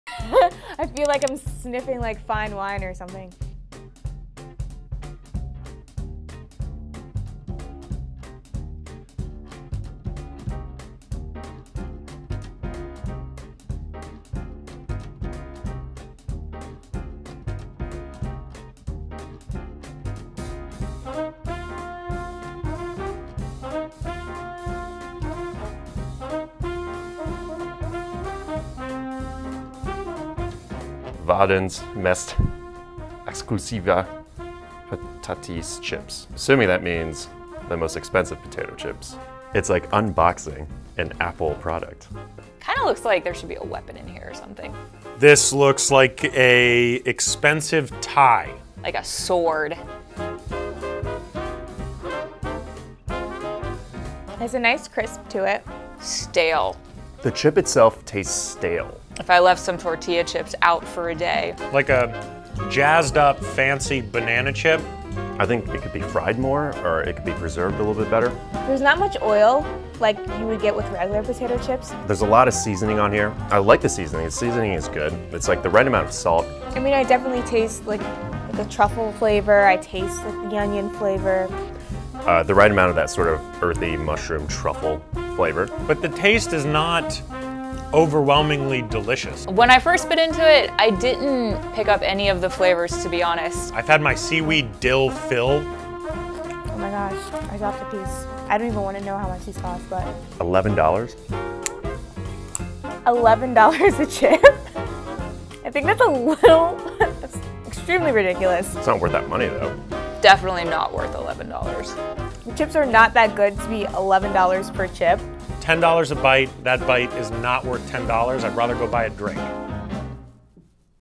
The world's most expensive potato chip
Swedish brewery St. Eriks wanted to create a snack to go along with it's carefully crafted IPA. Their search created this: the world's most exclusive, and expensive potato chip. Made with rare ingredients, the chips cost about $55 for a box of five. That's right, just five, single chips. But how do they taste? We put them to the test.